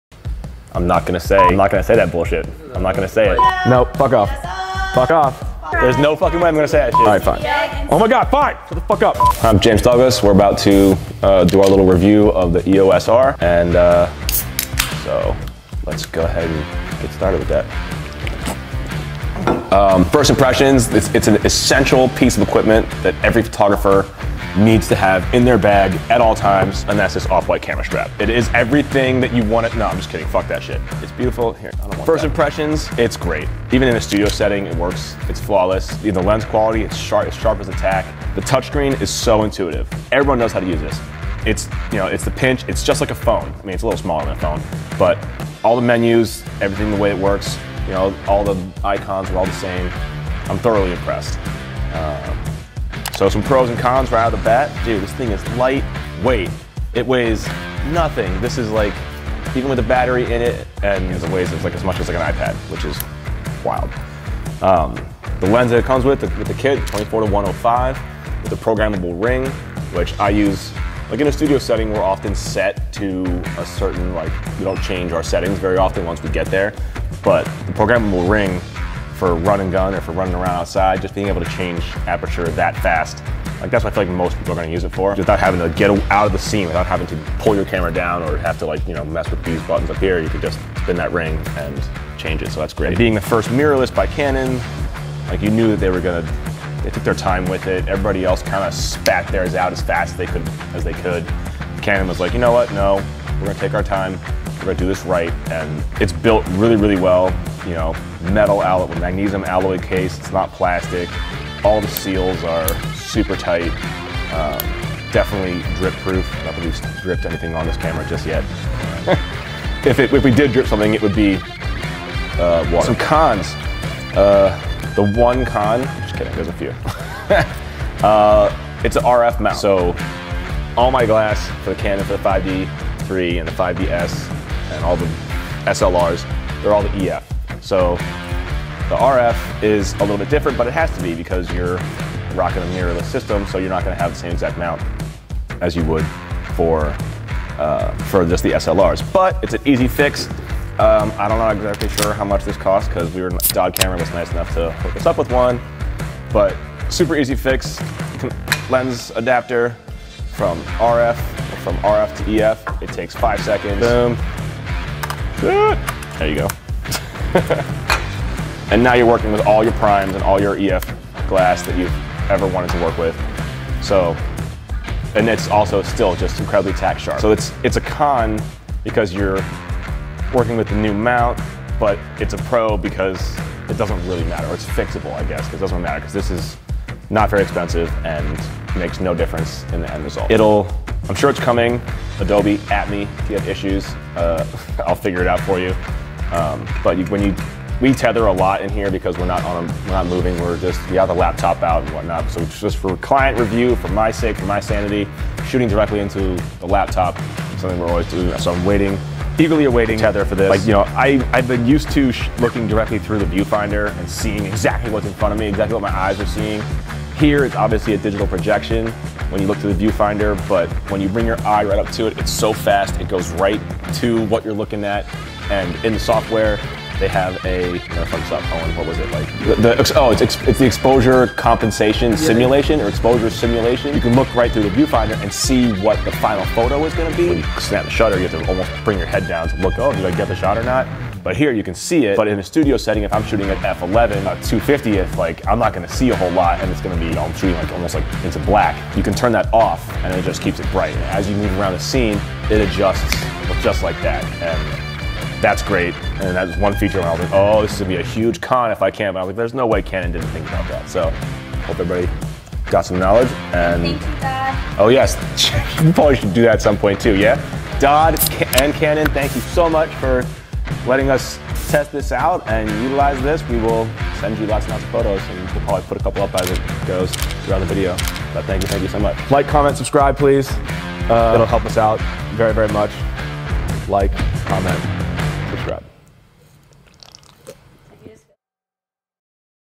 Canon EOS R REVIEW - The James Douglas Studio
Took a look at the brand new Canon EOS R with RF 24-105mm f4.0 kit! Thanks to Dodd Camera for hooking it up with all the sweet gear and making this video possible.

Featured Gear (via Dodd Camera):
- Canon EOS R Mirrorless Camera w/ 24-105mm RF Mount lens kit